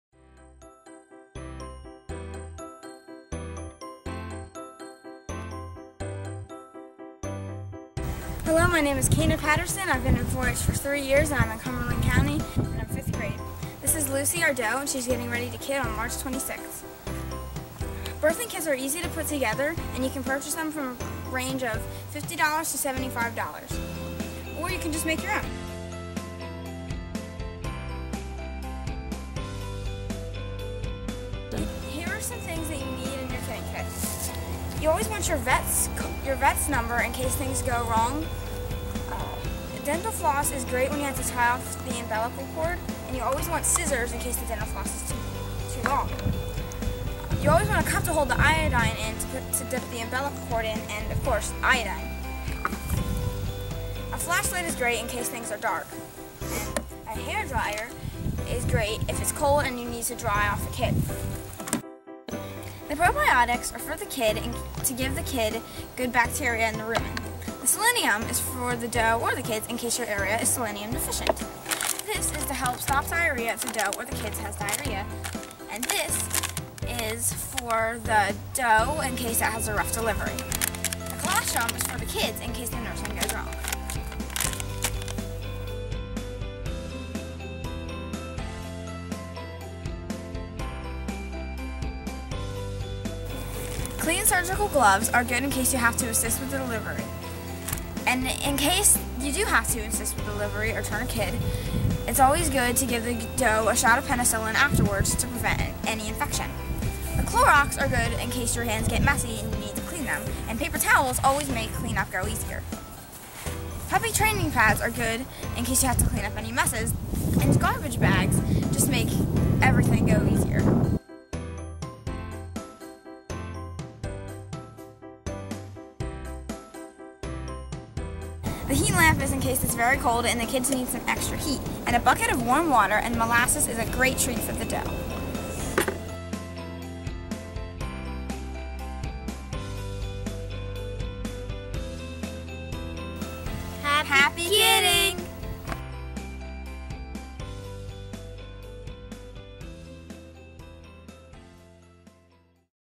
Goat Birthing Kit
Produced by New Jersey 4-H members (Cana Patterson and Geneva Patterson) enrolled in the sheep and/or goat projects as part of the 2017 NJ 4-H Sheep and Goat Video Contest.  One of several videos shared during the film festival of the Jr. Breeder Livestock Symposium at Rutgers.